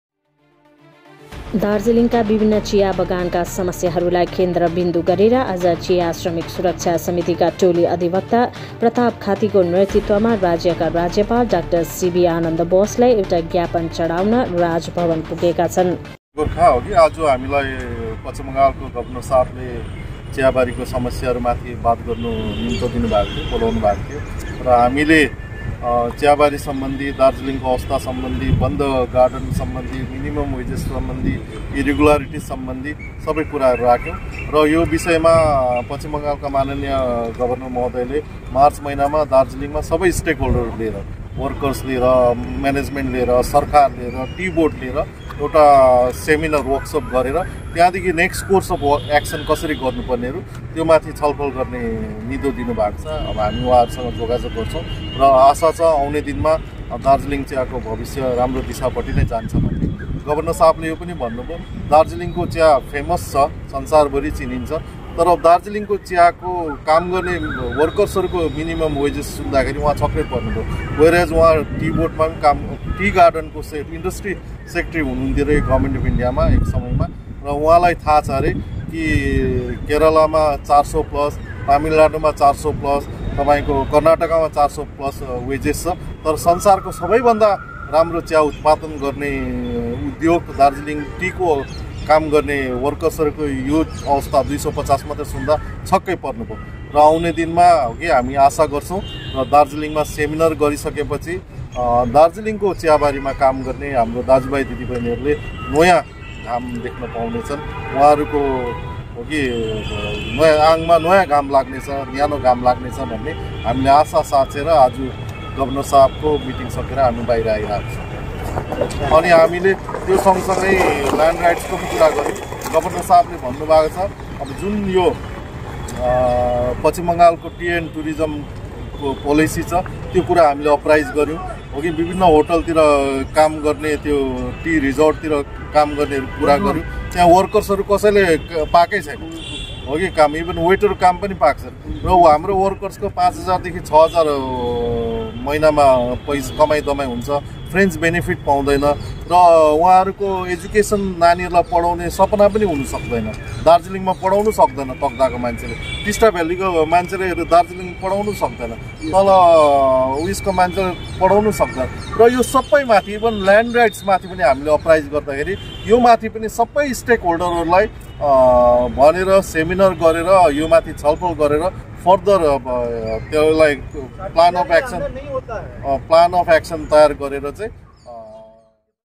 दार्जीलिङका विभिन्न चियाबगानका समस्याहरूलाई केन्द्रबिन्दु गरेर आज चिया श्रमिक सुरक्षा
दार्जीलिङका विभिन्न चियाबगानका समस्याहरूलाई केन्द्रबिन्दु गरेर आज चिया श्रमिक सुरक्षा समितिका टोली अधिवक्ता प्रताप खाातिको नेतृत्वमा राज्यका राज्यपाल डा° सीवी आनन्द बोसलाई एउटा ज्ञापन चडाउन राज भवन पुगेका छन्।